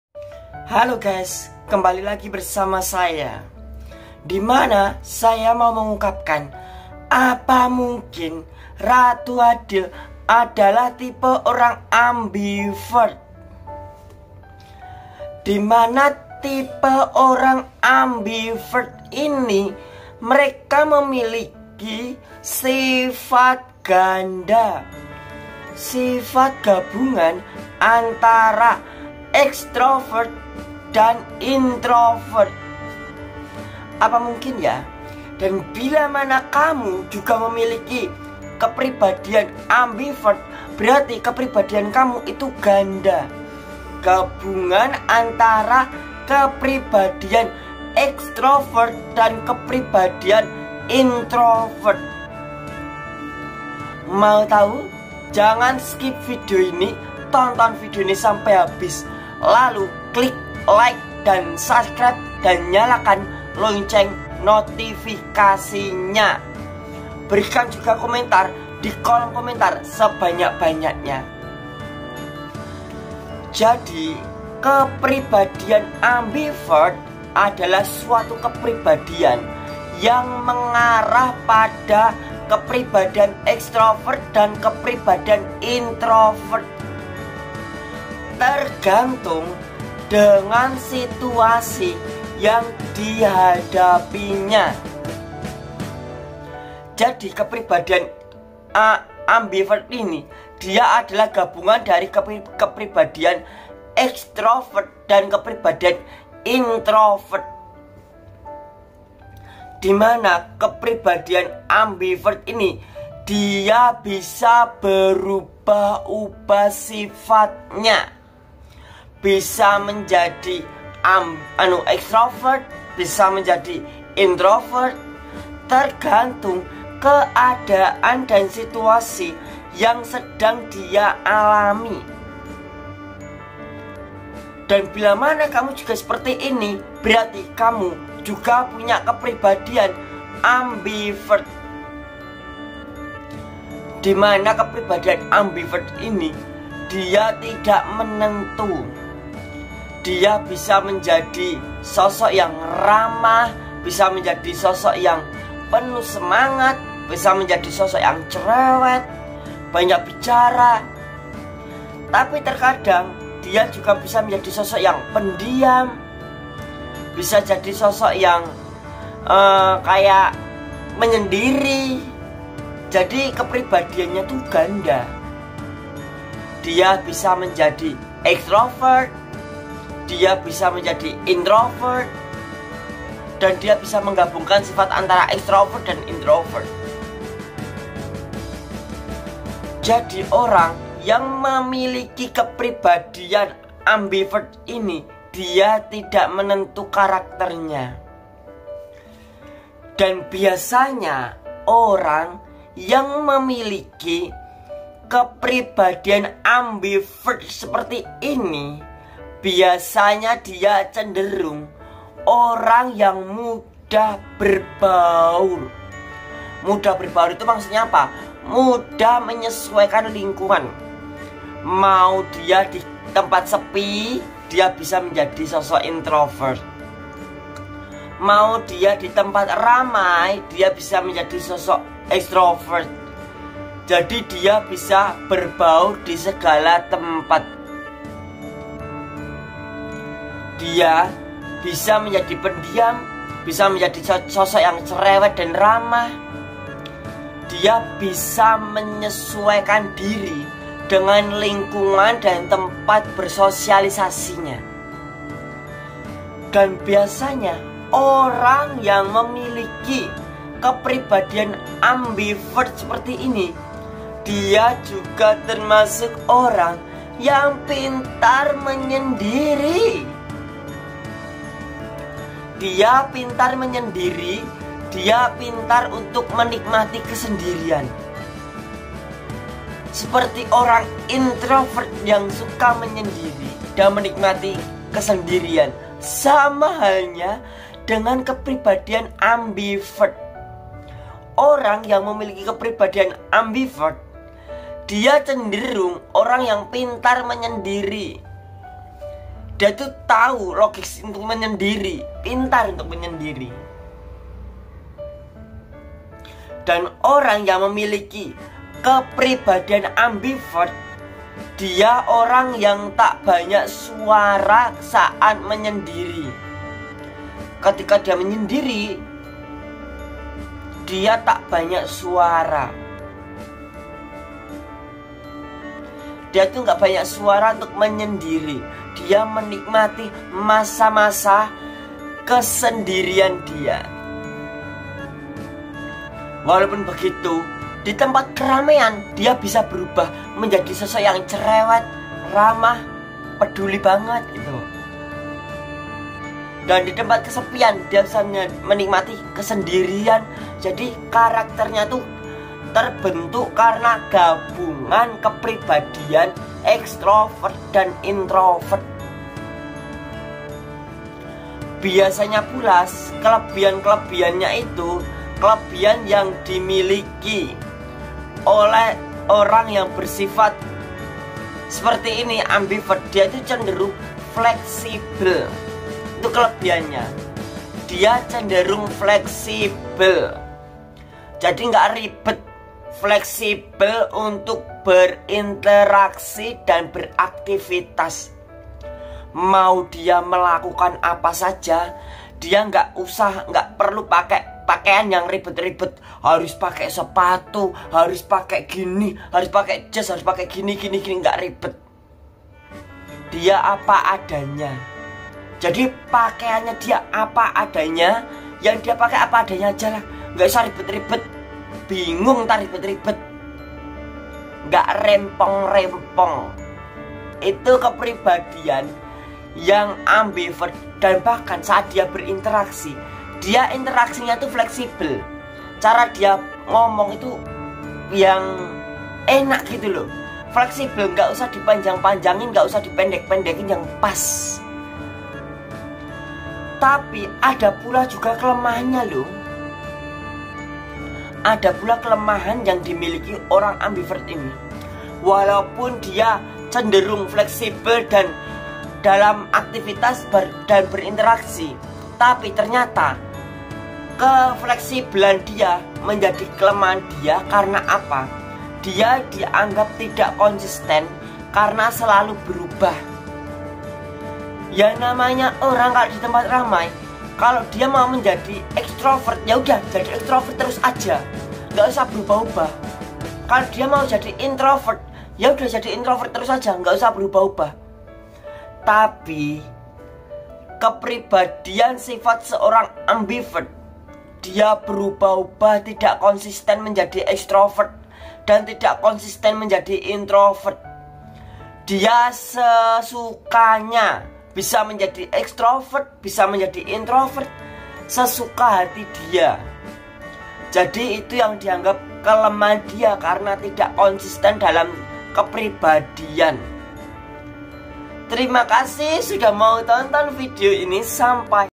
Apa mungkin Ratu Adil Ambivert‼️Gabungan Kepribadian#ratuadil
Vidio ini menjelaskan mengenai ratu adil yang merupakan seorang dengan kepribadian ganda.

Semoga bermanfaat.